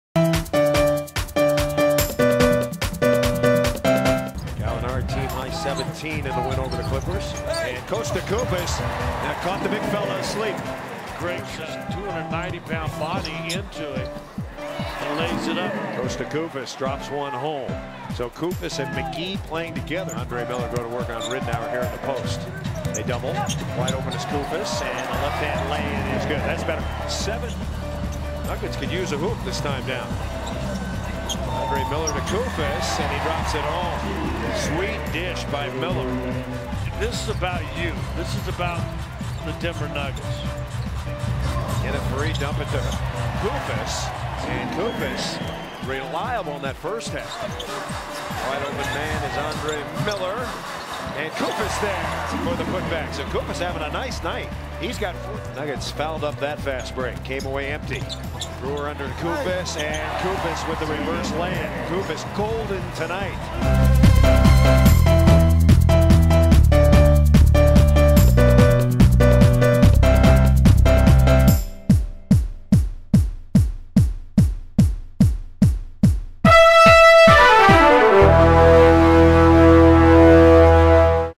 Kosta Koufos vs. Minnesota Timberwolves
Highlights of Denver Nuggets Center Kosta Koufos against the Minnesota Timberwolves.